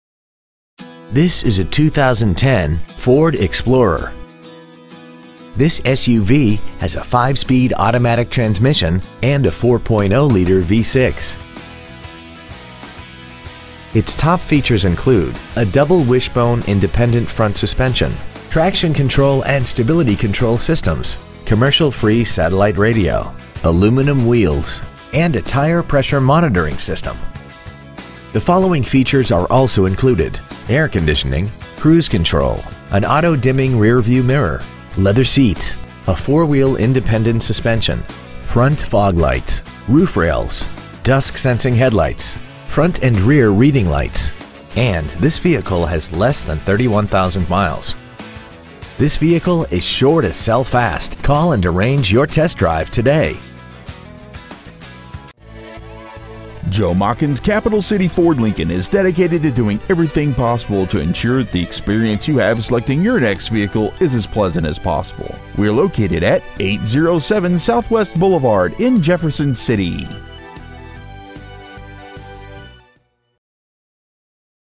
2010 FORD EXPLORER Jefferson City MO
Year : 2010
 Make : FORD
 Model : EXPLORER
 Engine : 4.0L V6 12V MPFI SOHC
 Trans . : 5-SPEED AUTOMATIC
 Exterior : BLACK
 Miles : 30,908

 Stock : E8086

 807 Southwest Blvd.